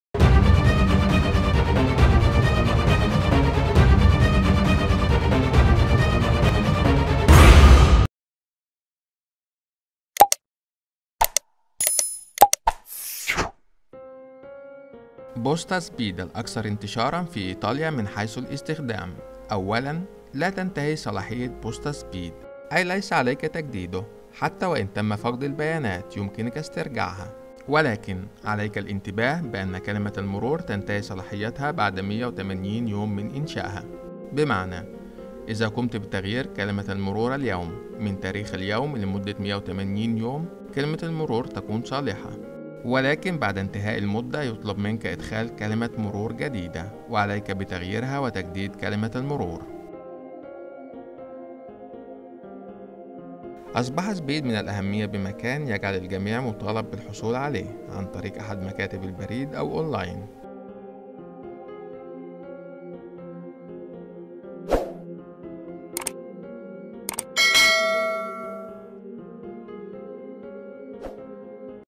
ماهى مدة صلاحية SPID POSTE؟
SPID POSTE
الاكثر انتشارا فى ايطاليا من حيث الاستخدام
اصبح من الاهمية بمكان , يجعل الجميع مطالب بالحصول عليه عن طريق احد مكاتب البريد او اون لاين

يمكنك ايضا الاطلاع على